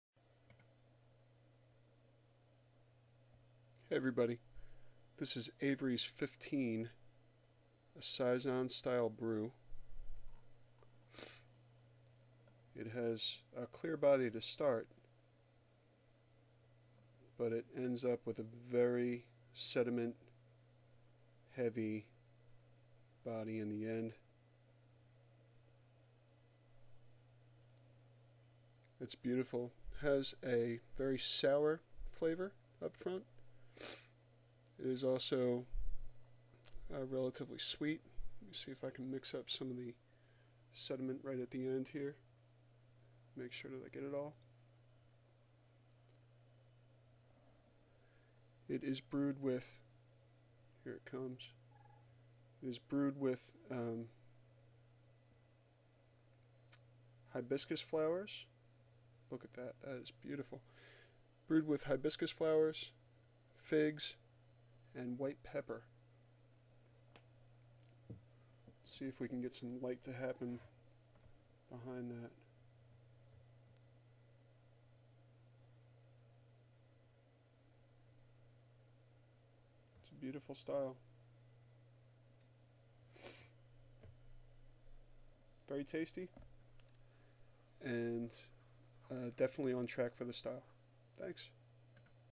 Pourn: Avery Fifteen Saison
Avery Fifteen is a golden bodied, white headed farmhouse style brewed with figs, hibiscus flowers, and white pepper. Funky, fruity, sour, and bitter, it is a surprising ale with a fantastic and generous dark finish.